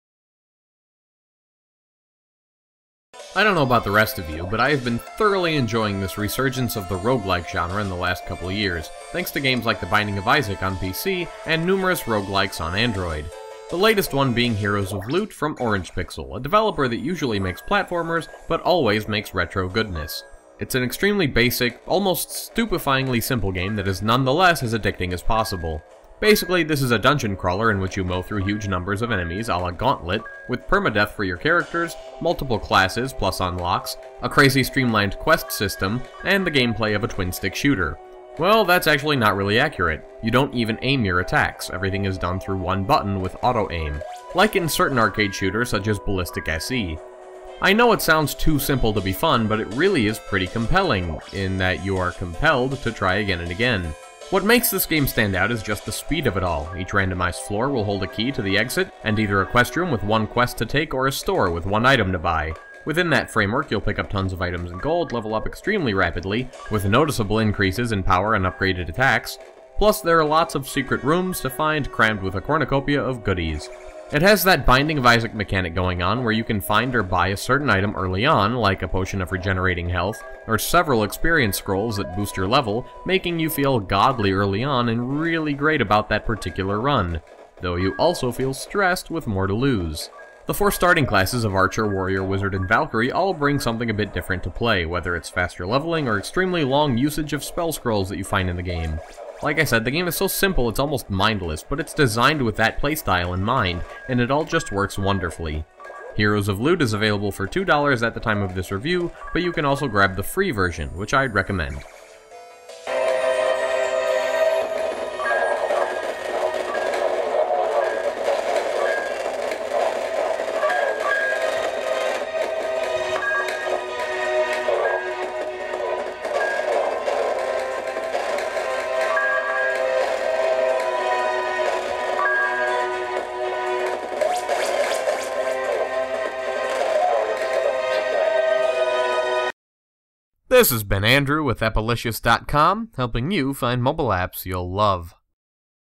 Heroes of Loot Android App Review - AndroidApps.com
Today: A great dungeon crawling roguelike time slayer.

App: Heroes of Loot
Price: $1.99
The price depicted is at the time of this review, and could easily be changed.